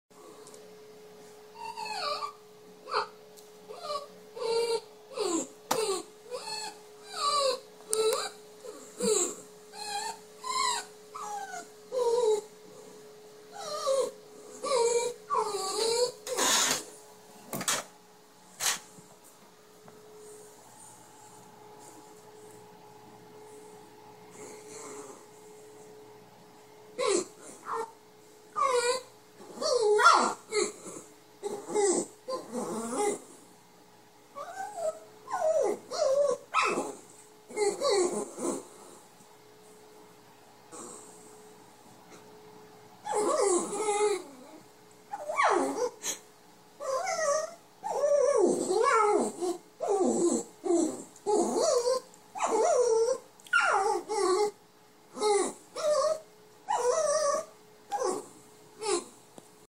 Paulie chit chat
Paulie talks and begs for a bone!